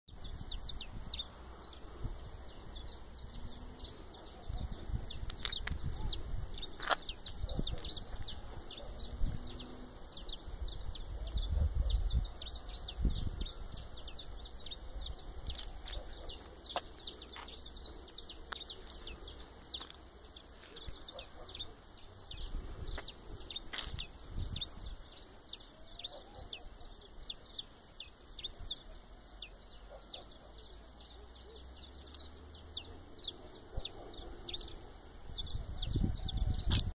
Amanhecendo o dia em minha caminhada matinal. Canários da terra saúdam o novo dia...
Amanhecendo o dia em minha caminhada matinal em Resplendor MG. Canários da terra saúdam o novo dia...